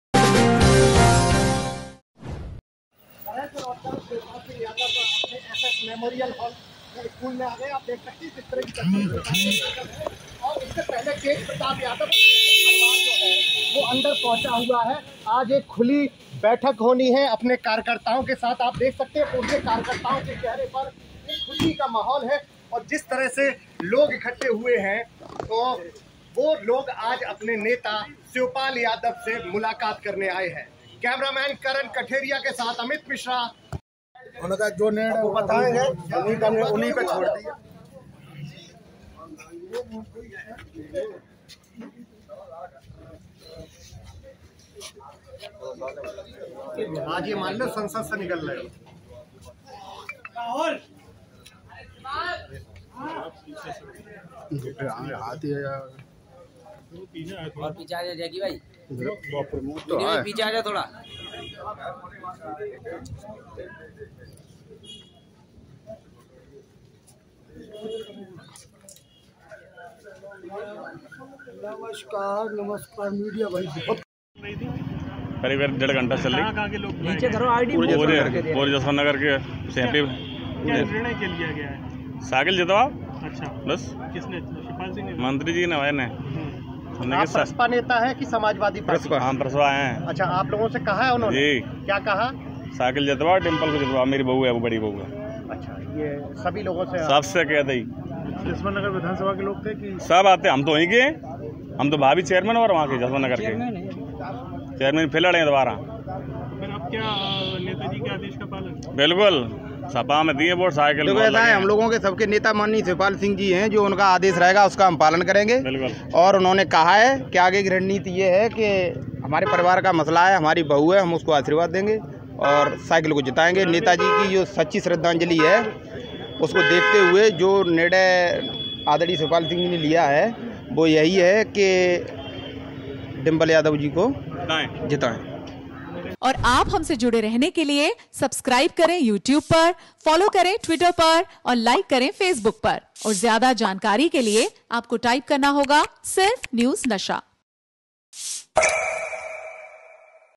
Shivpal Singh Yadav ने कर दिया ऐलान, BJP का लगा तगड़ा झटका ! Newsnasha
Shivpal Singh Yadav announced, BJP got a big blow! Newsnasha